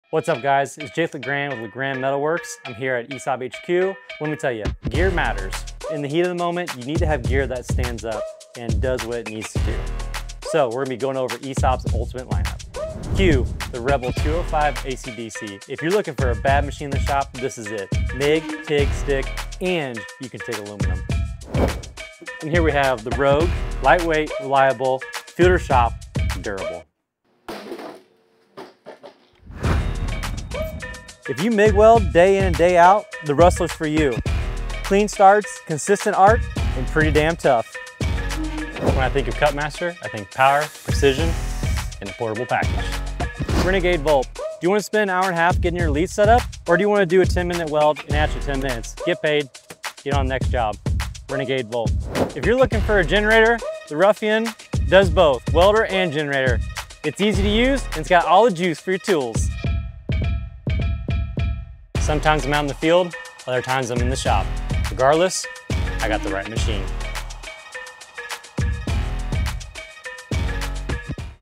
Choosing Your Next Welder with @LeGrandMetalWorks⁩ | ESAB’s Ultimate Lineup of Welding Equipment
What’s the best welding machine for your setup? Jake LeGrand (@LeGrandMetalworks) puts ESAB’s full lineup to the test—covering everything from portable powerhouses to shop-floor workhorses.

Here’s what we cover:

Rebel EMP 205ic AC/DC – Multi-process MIG/TIG/Stick with sMIG intelligence (00:12)
Rogue – Lightweight Stick/TIG built tough for the field (00:21)
Rustler EM 280 Pro – Production-ready MIG machine (00:29)
Cutmaster+ – Plasma cutting precision with serious power (00:38)
Renegade VOLT – Battery-powered TIG/Stick with no cords required (00:43)
Ruffian – Engine-driven welder + generator combo for the roughest sites (00:52)
Outro – (01:02)

From shop floors to off-grid welding, ESAB has a solution built for the way you work.

👉 Which machine would you put to work in your shop or jobsite? Drop your pick in the comments!